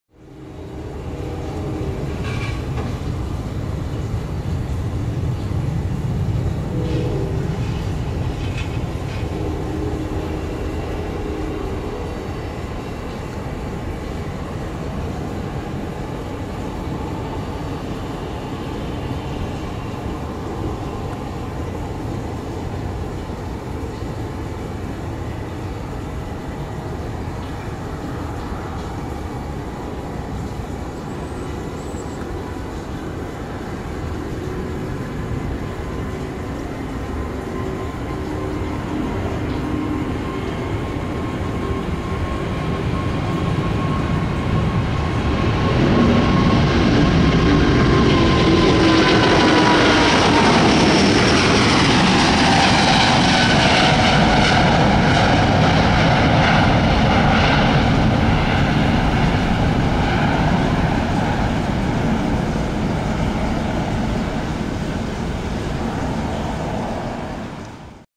Delta A350-900 take off, DTW
Delta Air Lines Airbus A350-941 N503DN departing from Detroit metro, DTW runway 22L 9-7-23. Flt DAL132 to AMS